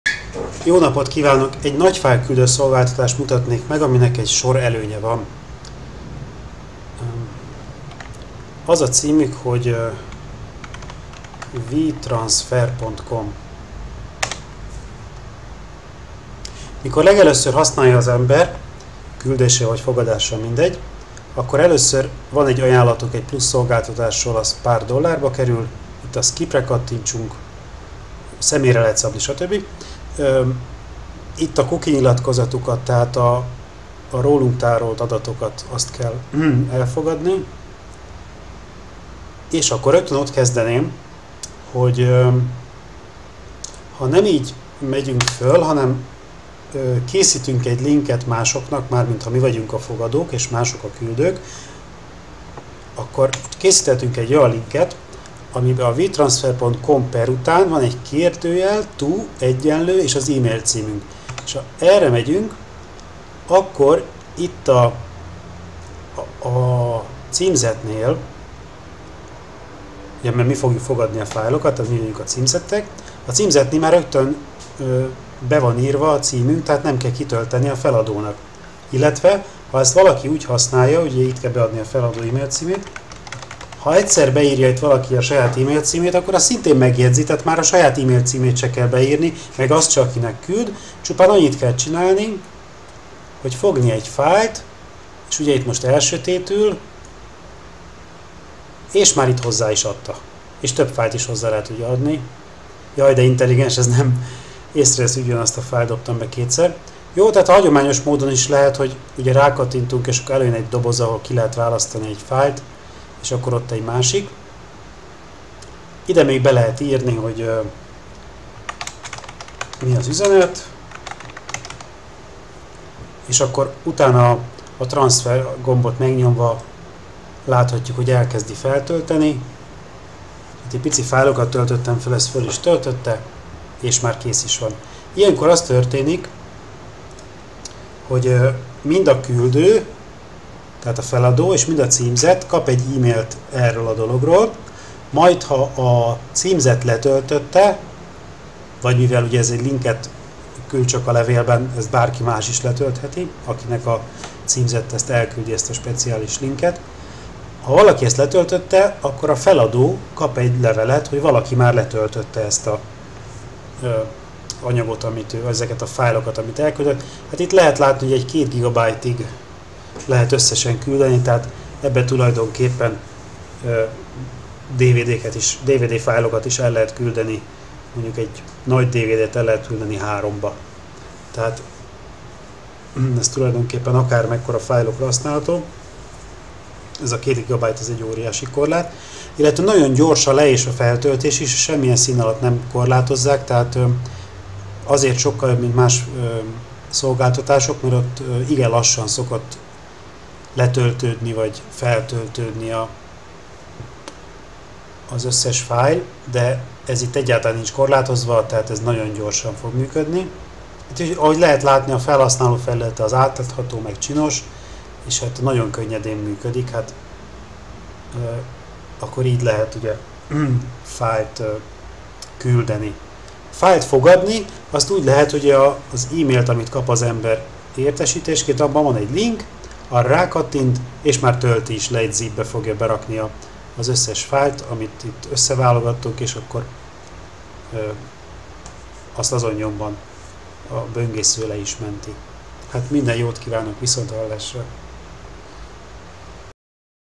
WeTransfer használata nagyfájl küldés-fogadás
A wetransfer.com nagyfájl küldő szolgáltatás sok előnnyel jár: könnyű használat, nagy le- és feltöltési sebesség, kedves felhasználói felület, email-es értesítés.